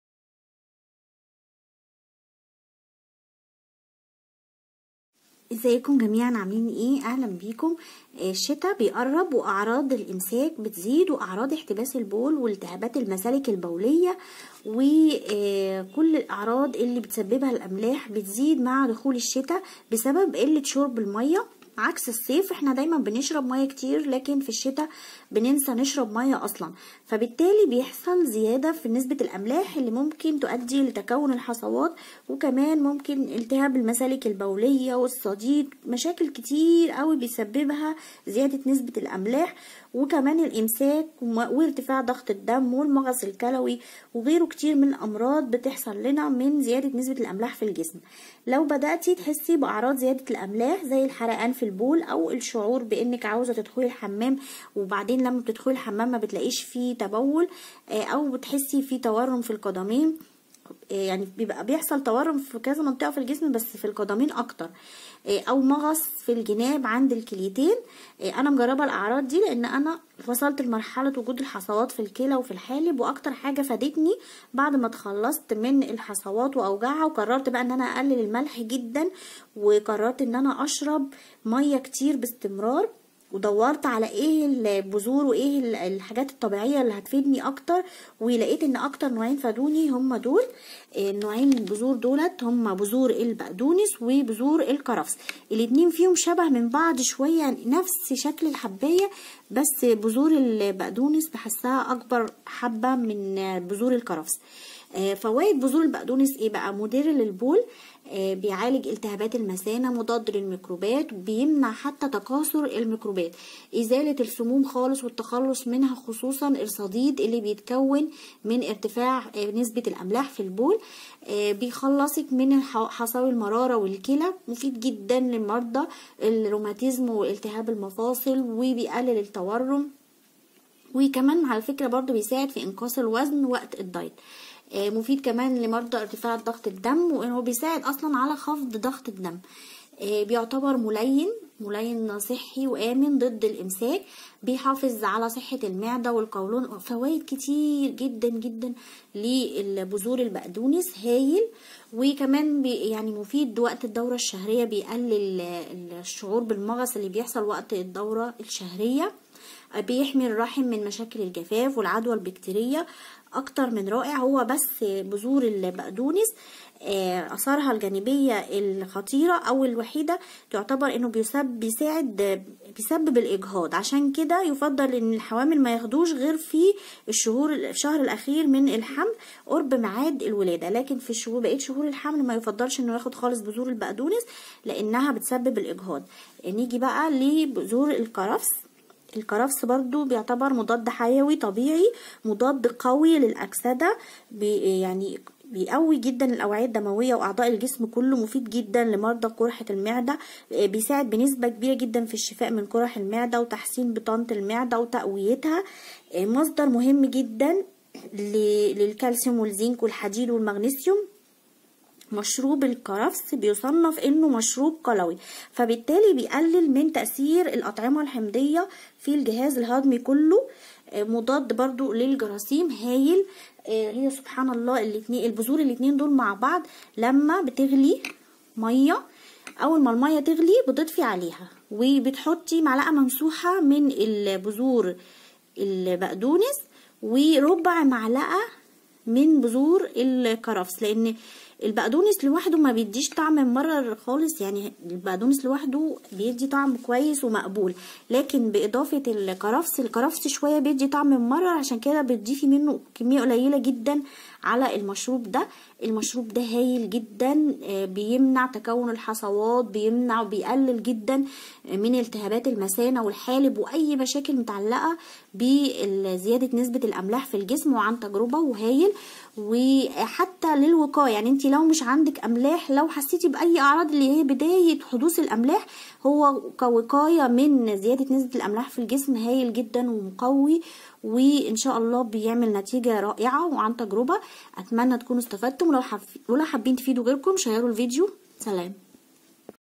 الشتاء يعنى إمساك يعنى زيادة الاملاح عشان كدة لازم تتعرفو على طريقة اخذ بذور البقدونس 👌
اهم مشروب لعلاج الامساك و زيادة الاملاح فى الجسم